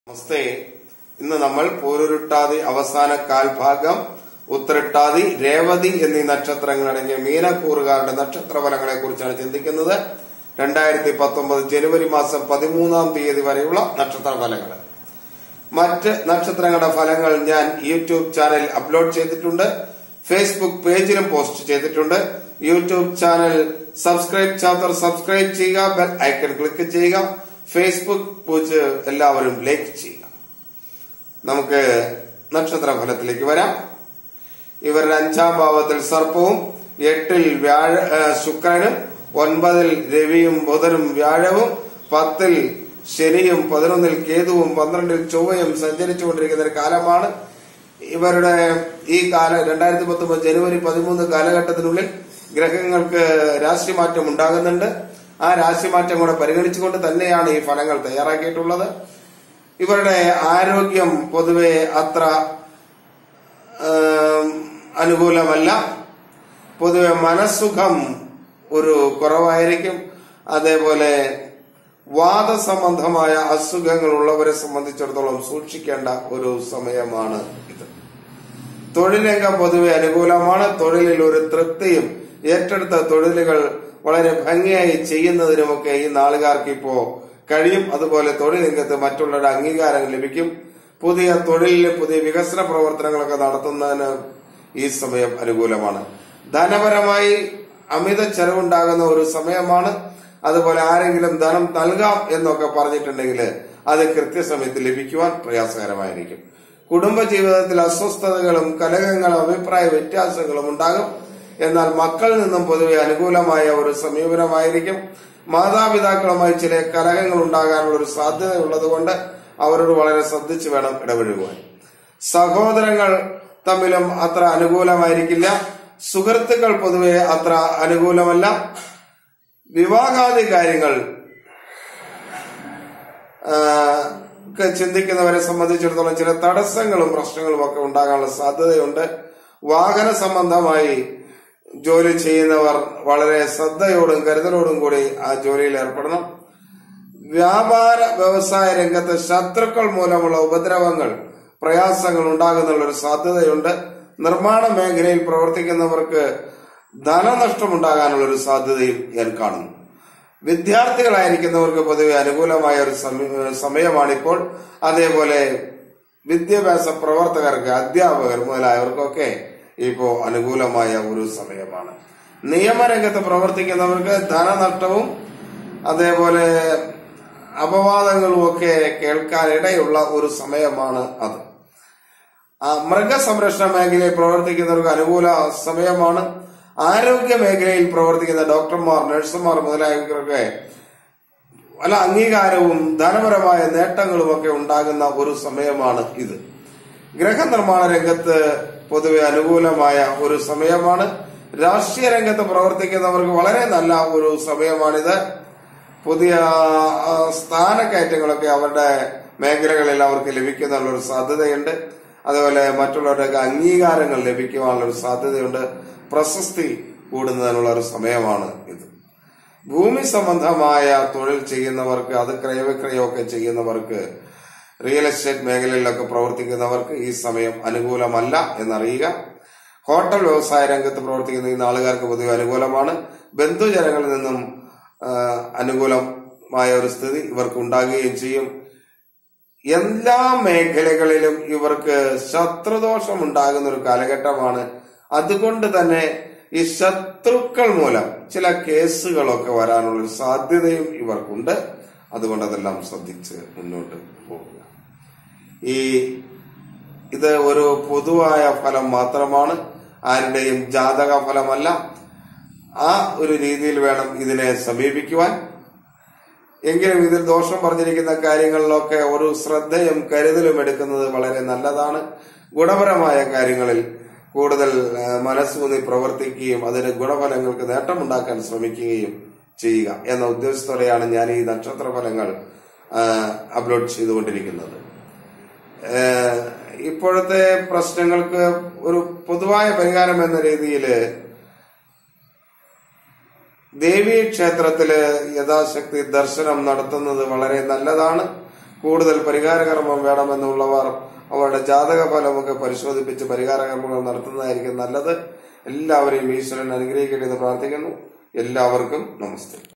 പൂരുരുട്ടാതി , ഉത്തൃട്ടാതി , രേവതി നക്ഷത്രഫലംAstrologer & vasthu consultant P. V. A.Nair,
പൂരുരുട്ടാതി   ,  ഉത്തൃട്ടാതി , രേവതി നക്ഷത്രഫലം

 13 ജനുവരി    2019  വരെ

Astrologer & vasthu consultant  P. V. A.Nair, Odakkali jyothisham, Odakkali, perumbavoor, Ernakulam Dist, ph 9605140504